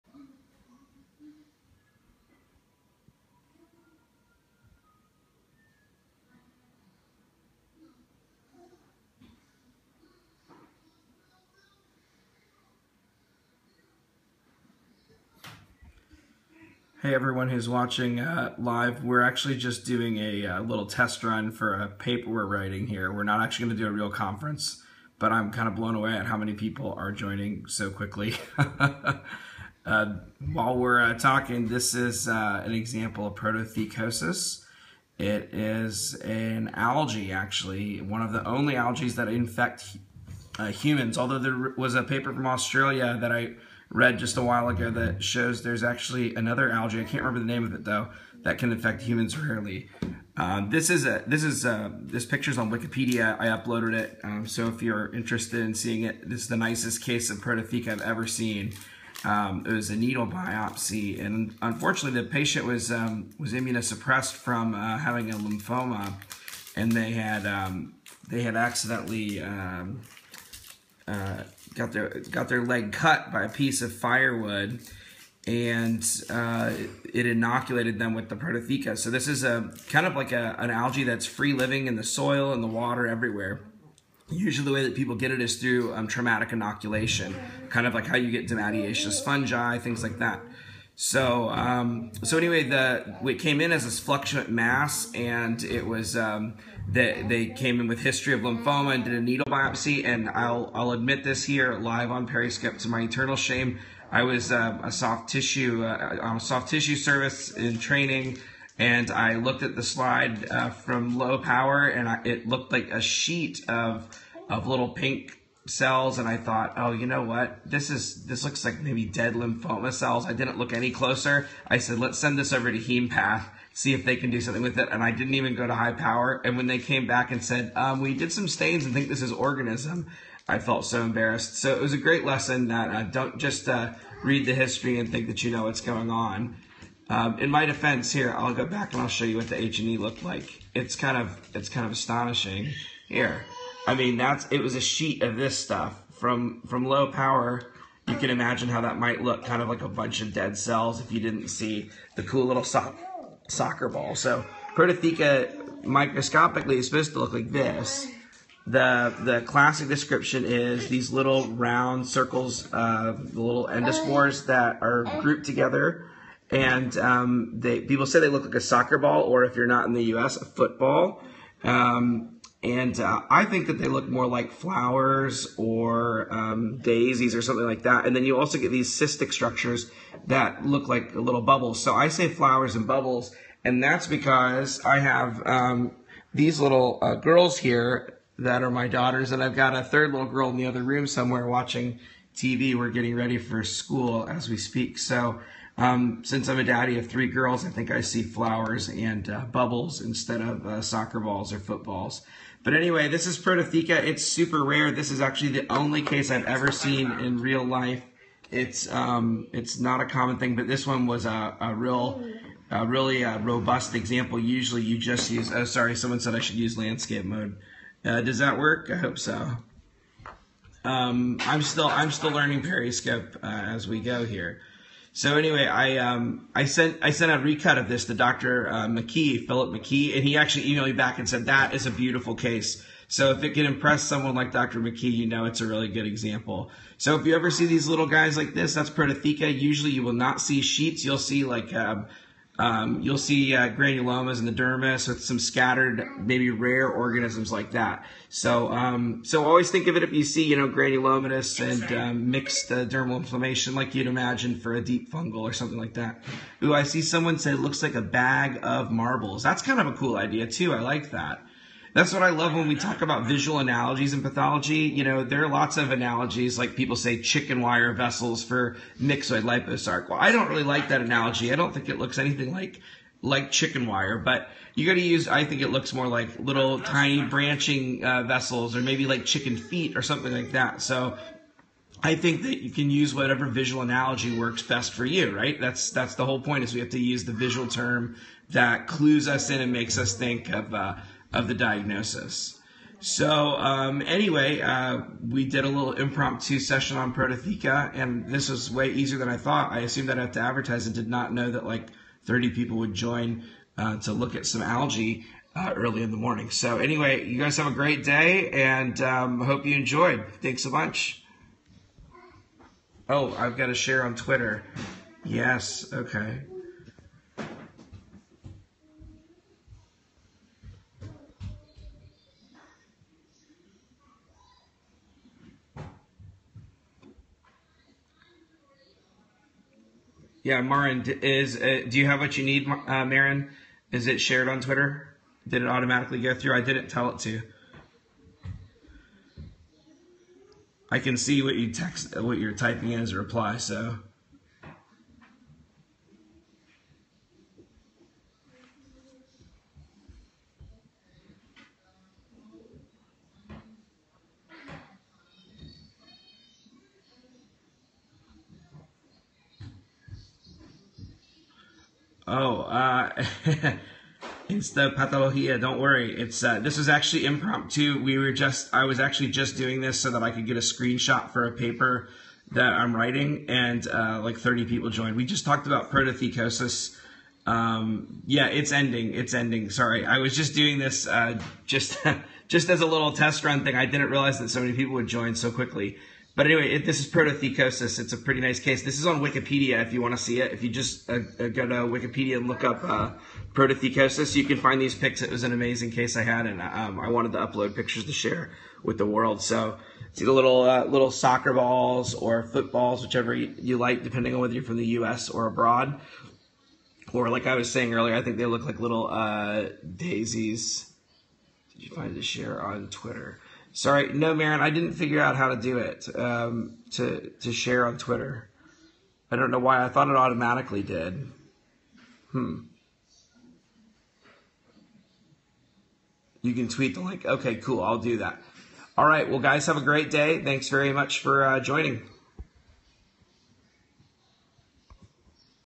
Protothecosis (an algae that infects humans)
Protothecosis is a rare infection in both animals and humans. It is very unusual because it is one of the only human infections caused by an algae (a singled celled plant), Prototheca wickerhamii. The idea of plant cells being able to infect humans still seems so amazing to me. This was an impromptu Periscope broadcast I did on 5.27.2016. I had intended to only broadcast for a minute so I could obtain screenshots for a paper we (Maren Fuller, Sanjay Mukhopadhyay, and I) were writing about Periscope and pathology education (edit: here's the published paper Fig 1 shows screenshot of this video...so meta! LOL). I wasn't intending for anyone to watch. Amazingly, dozens of viewers started watching within seconds of broadcasting, maxing out at 51 viewers at the peak and over 200 live viewers for the whole session! This fortunate and unplanned incident further convinced me of the amazing power that Periscope holds for pathology education.

The other algae that can very very rarely infect humans is Chlorella